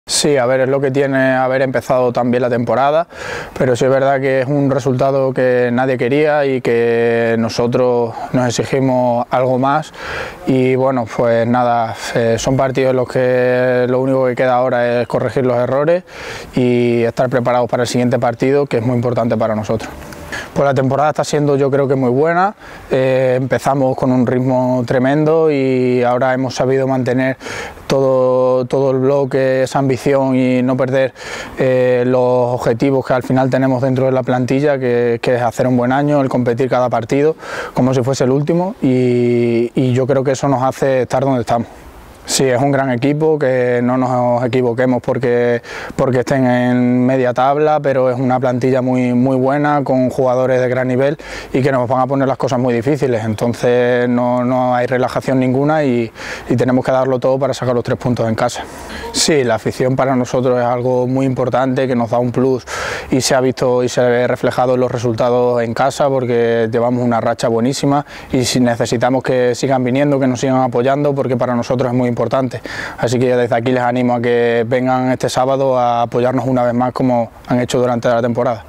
Declaraciones de Jesús Clemente (01/12/2022)
Jesús Clemente, jugador del CD Eldense, habla en la semana del partido contra la UD Logroñés.